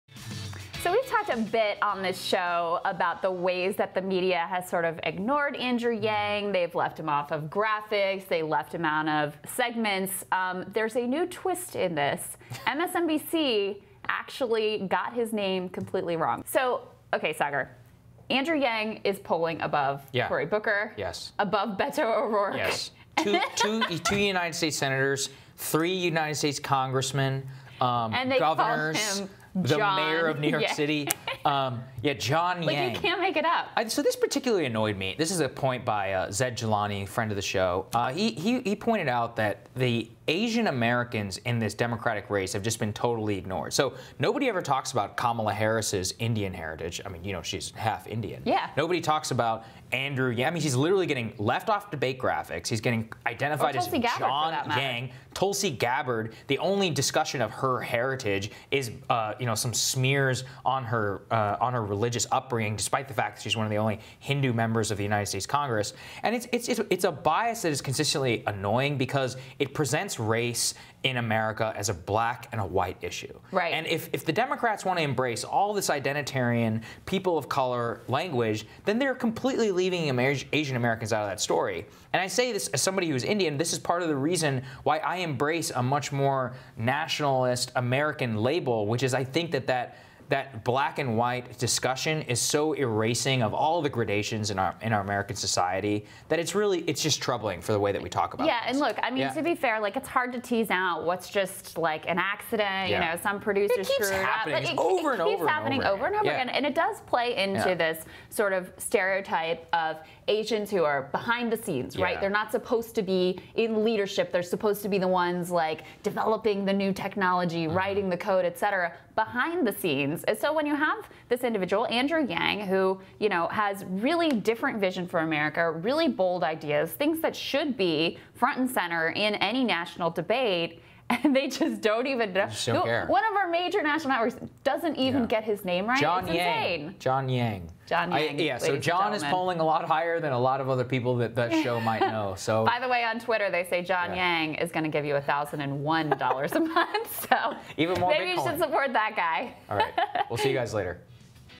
Who is John Yang? MSNBC Invents New Candidate
MSNBC invents new Presidential contender, "John" Yang.